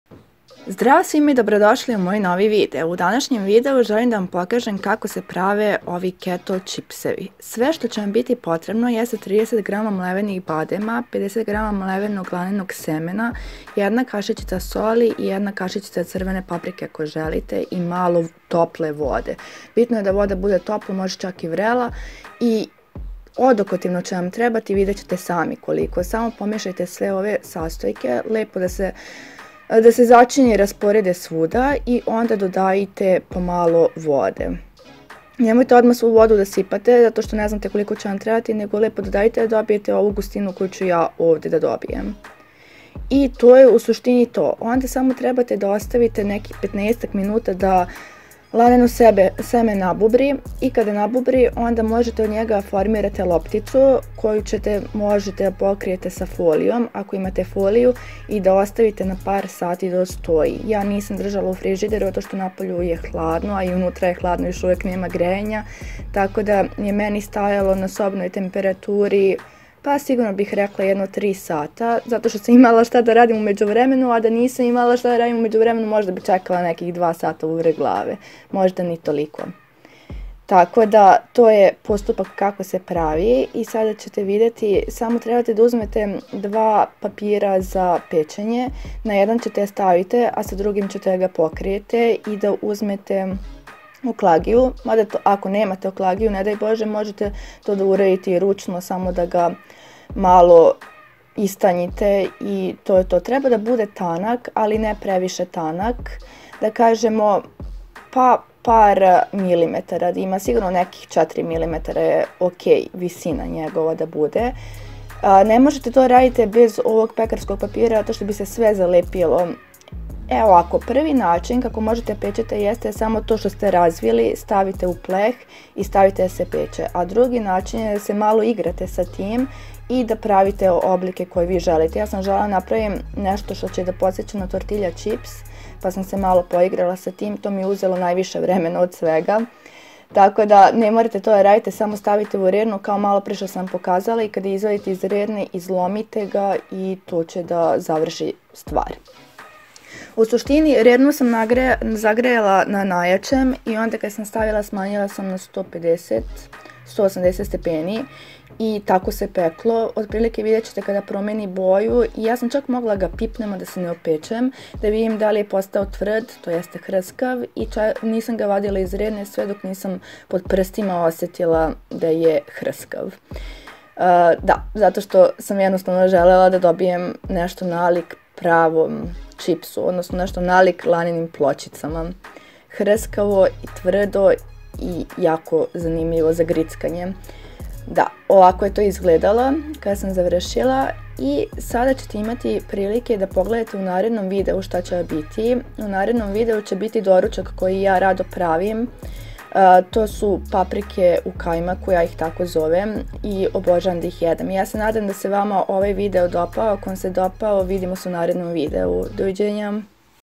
KETO CHIPS 2 - LOW CARB ISHRANA
Sve sto vam je potrebno:
30g mlevenog badema
50g mlevenog lana
zacini po zelji

Ovde mozete pogledati prethodni keto chips od samo jednog sastojka

INSTAGRAM: @stefany steph   & @keto dnevnik ishrane